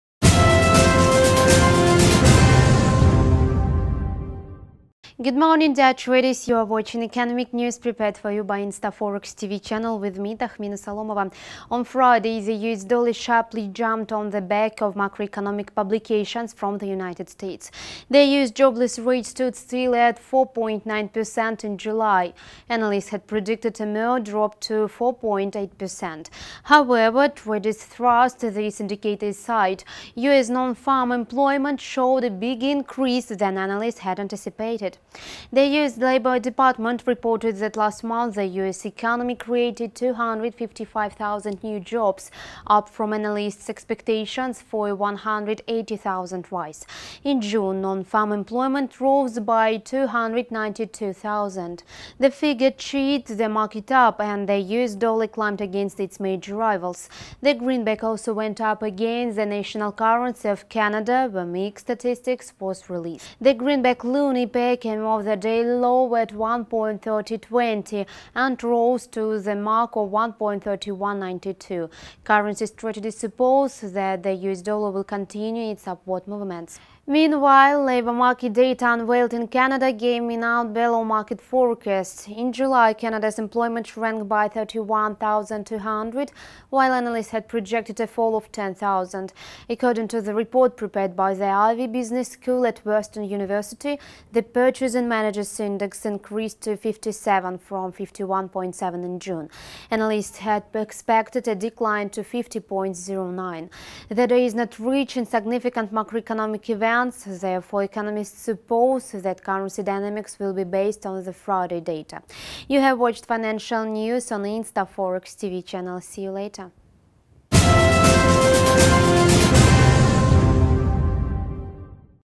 Greenback gains momentum on US labor market data
On Friday, the US dollar sharply jumped on the back of macroeconomic publication from the United States.
The US jobless rate stood still at 4.9 percent in July. Analysts had predicted a mere drop to 4.8 percent. However, traders thrust this indicator aside.
US non-farm employment showed a bigger increase than analysts had anticipated.
The US Labor Department reported that last month the US economy created 255,000 new jobs, up from analysts’ expectations for a 180,000 rise. In June, non-farm employment rose by 292,000.
The figure cheered the market up, and the US dollar climbed against its major rivals. The greenback also went up against the national currency of Canada, where mixed statistics was
The greenback-loonie pair came off the daily low at 1.3020 and rose to the mark of 1.3192. Currency strategists suppose that the US dollar will continue its upward movements.
Meanwhile, labor market data unveiled in Canada came in out below market forecasts.
In July, Canada’s employment shrank by 31,200 while analysts had projected a fall of 10,000.
According to the report prepared by the Ivey Business School at Western University, the purchasing managers’ index increased to 57 from 51.7 in June. Analysts had expected a decline to 50.09.
The day is not rich in significant macroeconomic events; therefore, economists suppose that currency dynamics will be based on the Friday data.